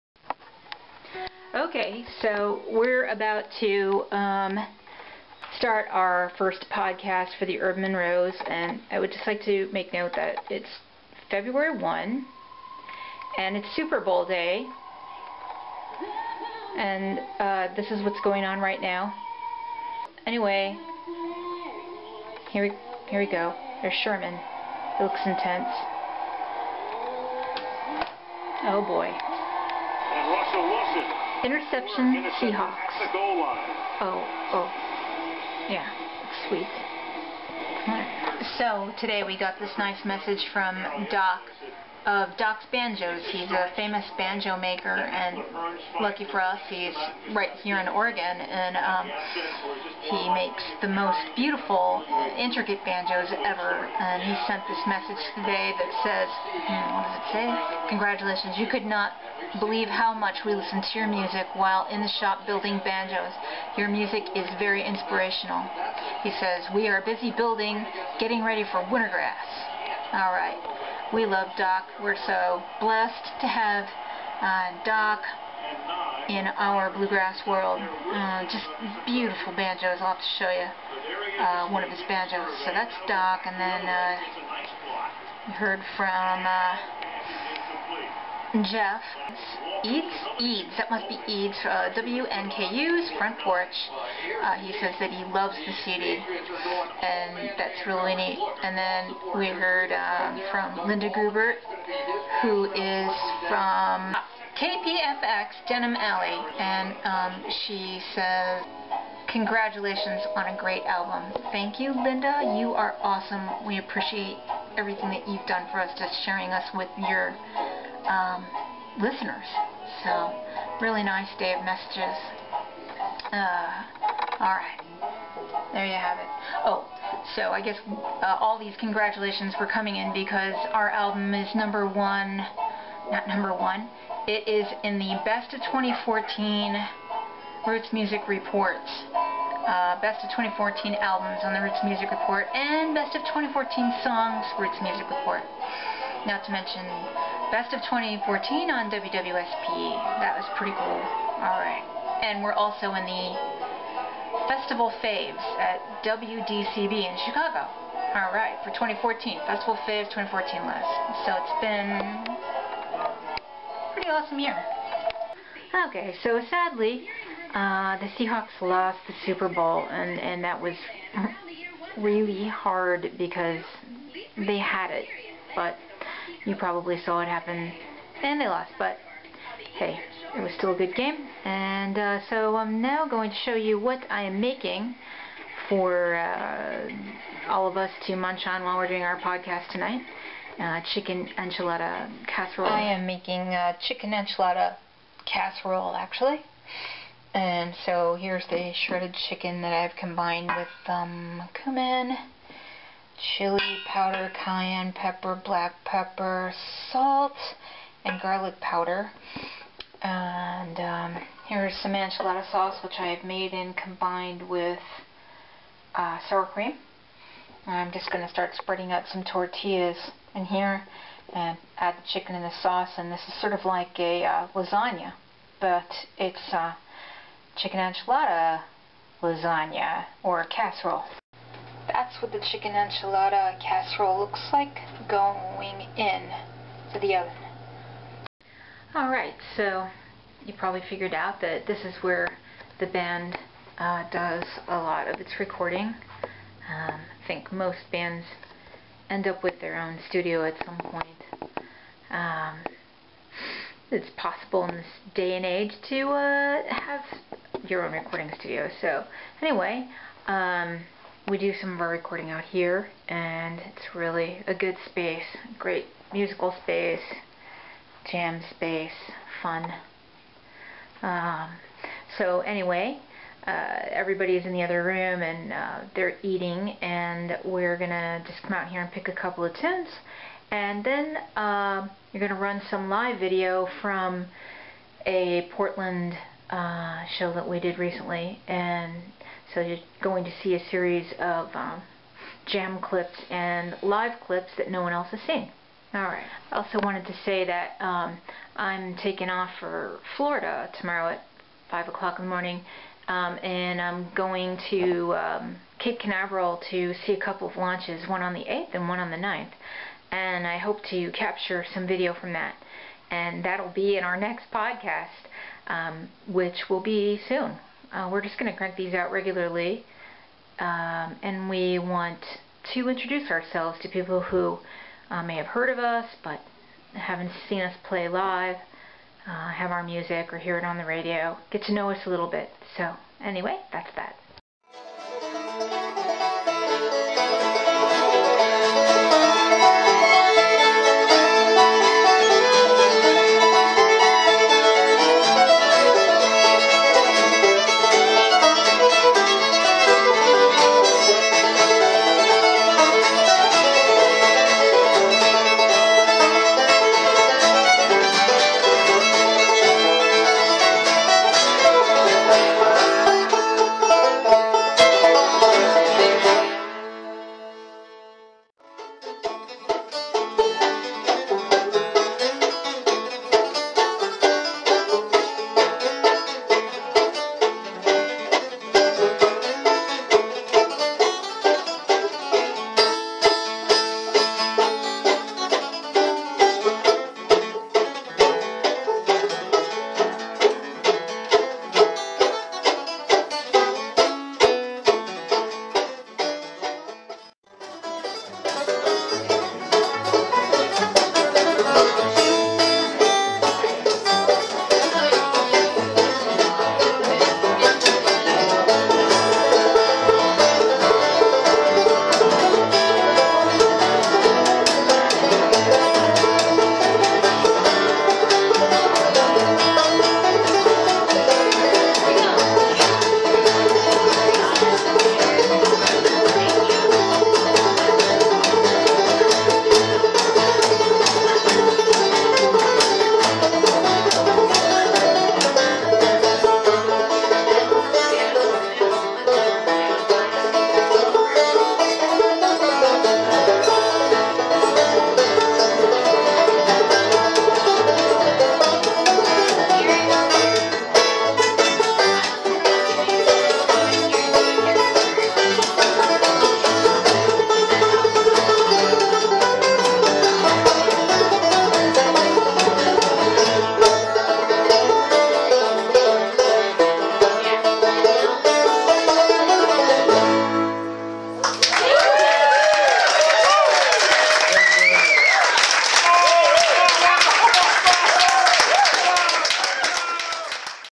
Urban Monroes Podcast Episode 1
Urban Monroes Podcast Episode 1: A personal view into our world featuring viewer mail, live podcast pickin', a demonstration of the making of chicken enchilada casserole for the band to eat at practice, a Seahawks Super Bowl interception, and a live clip from a recent Portland show.